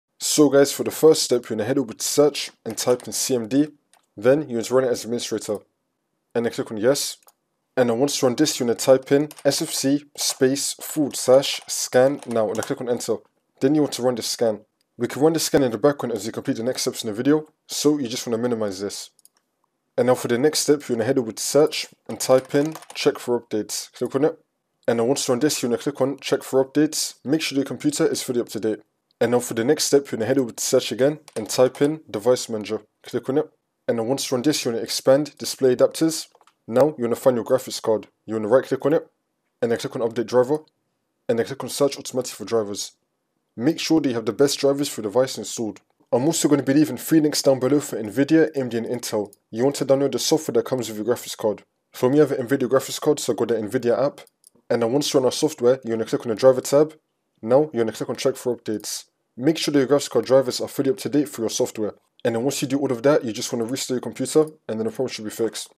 How To Fix Werfault.exe Application Error in Windows
In this video, I'll show you how to fix the "Werfault.exe Application Error" in Windows. This issue often occurs when the Windows Error Reporting tool encounters problems. I'll walk you through the steps to resolve this error and get your system running smoothly again.

WerFault is a Windows Error Reporting process in Windows 11 that helps various applications report issues to Microsoft. When an error occurs, WerFault collects information about the problem and attempts to find a solution, benefiting both the user and Microsoft. The application was unable to start correctly 0xc0000005 Click OK to close the application.

The WerFault.exe file is located in the System32 folder of your Windows 11 operating system and is responsible for handling Windows error reports.